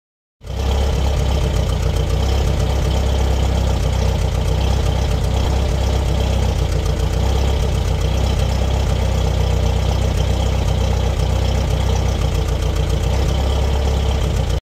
1995 Jeep yj idle issue
This started happening after I changed my sparkplugs, wires and distributor cap